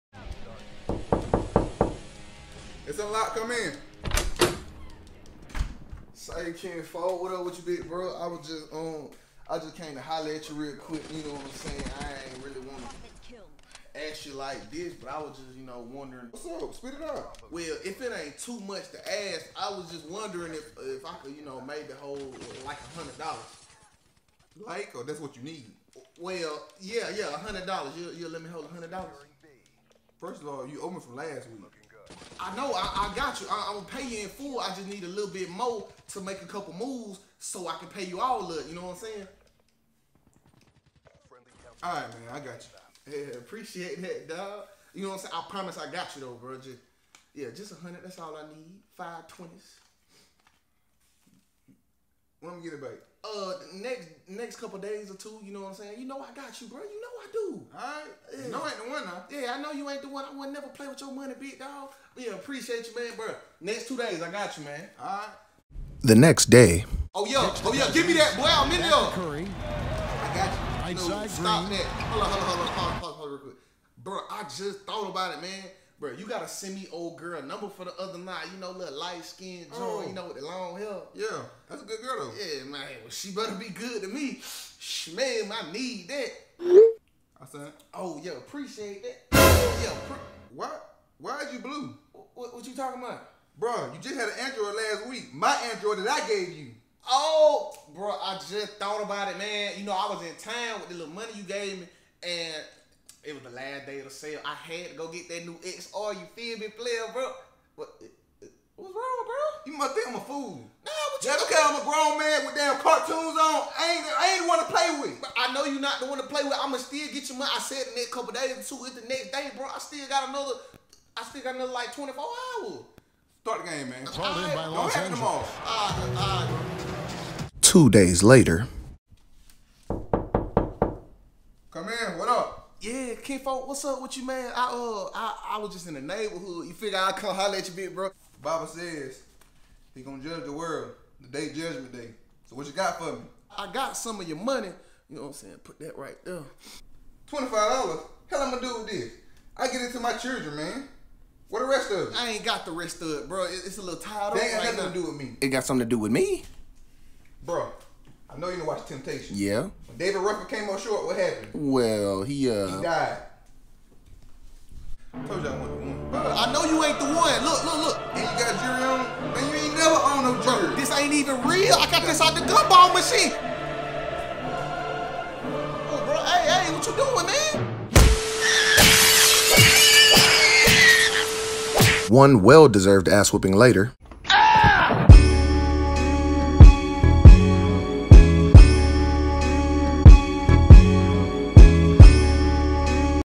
When Folks Play With Your Money
These leech ass folks be out here fresh as hell buying the newest stuff knowing damn well they owe you money. Lmao unc had something for that tho